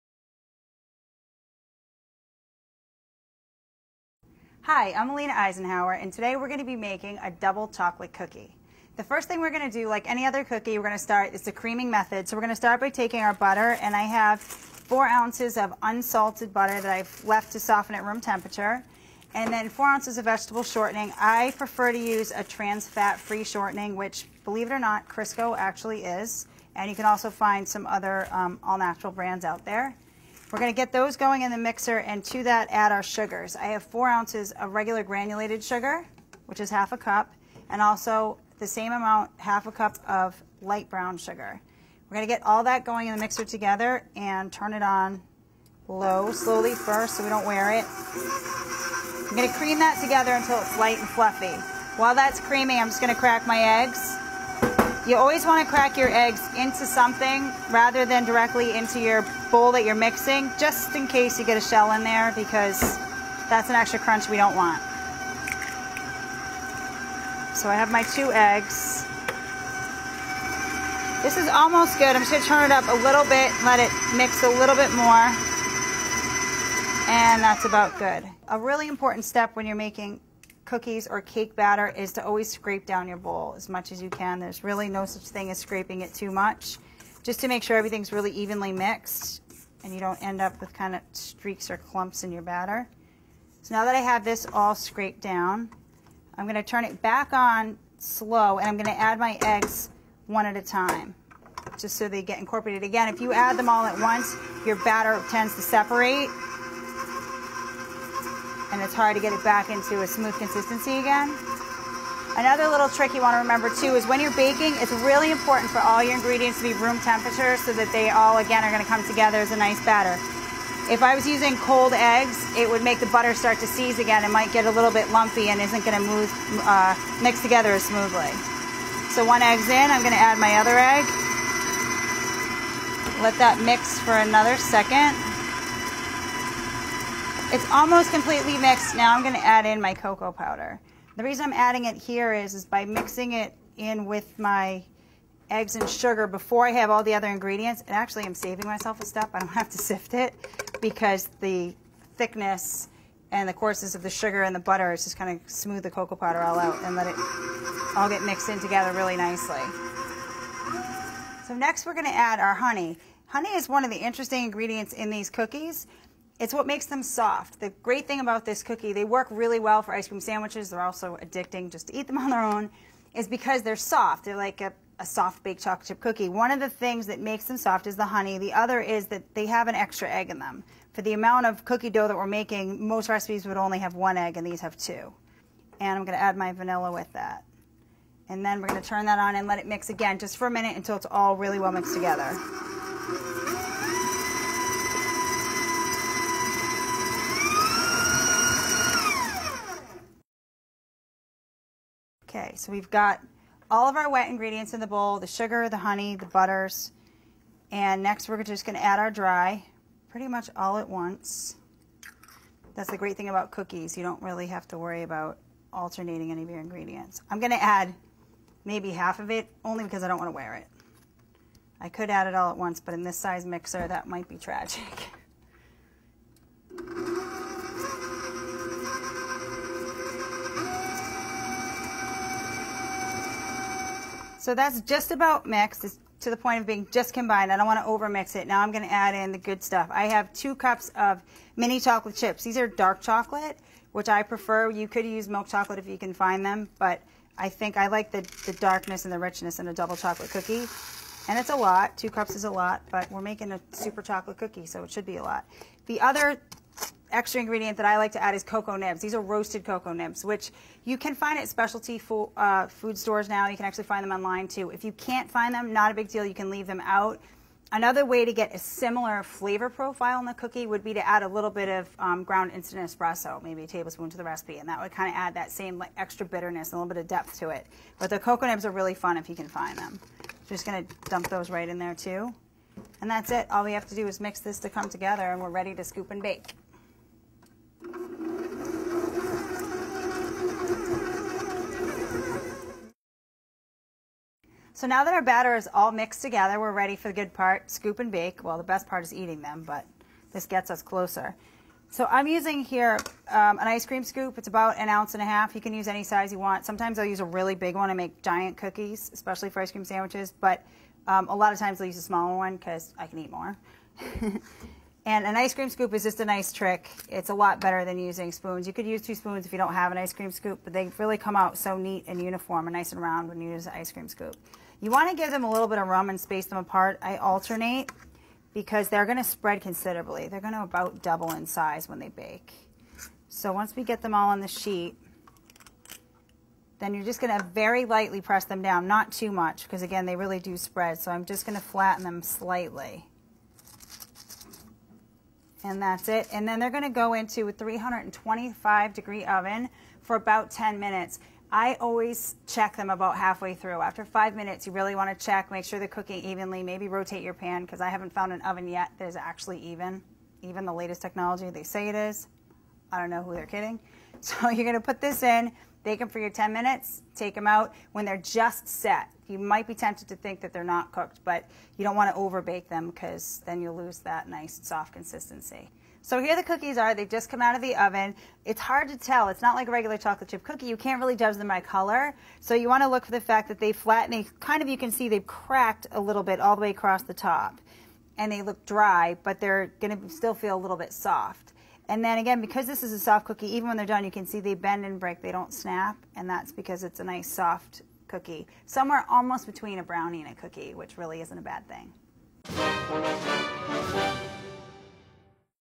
Soft Double Chocolate Cookie
Food Network's Sweet Genius winner Alina Eisenhauer of Sweet Bakery & Bar, has proven that she knows what she's doing when it comes to desserts. These soft chewy cookies with Dutch cocoa powder, chocolate chips and honey, are great on their own and also perfectly suited for ice cream sandwiches.